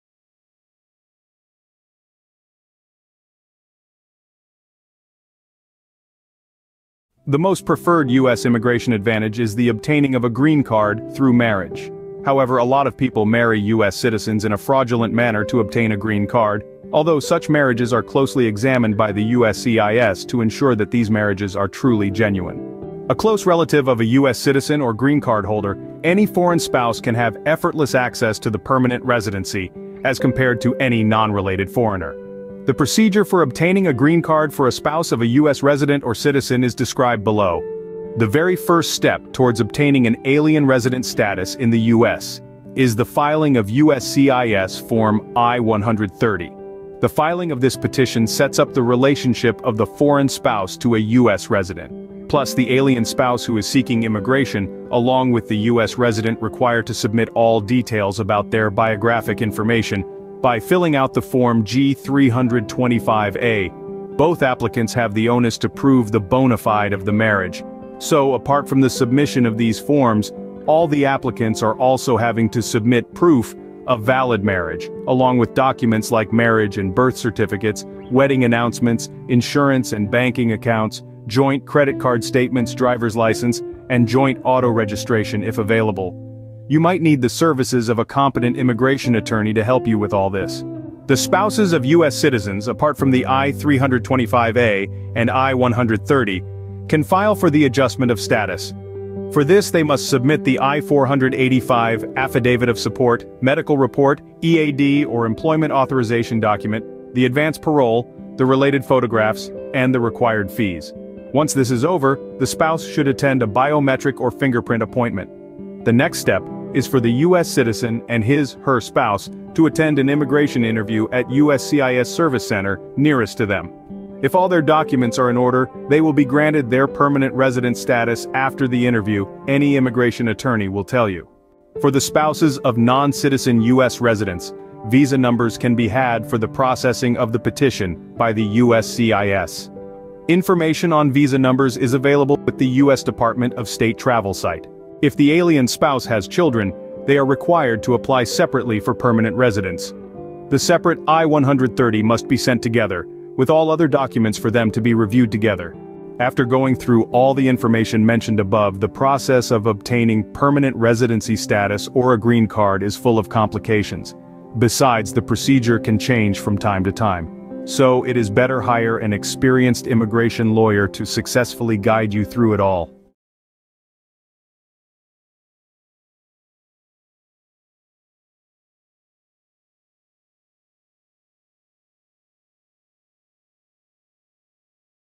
How to Get A US Green Card Through Marriage
A marriage or fiance visa is a sure way to obtain a US Green Card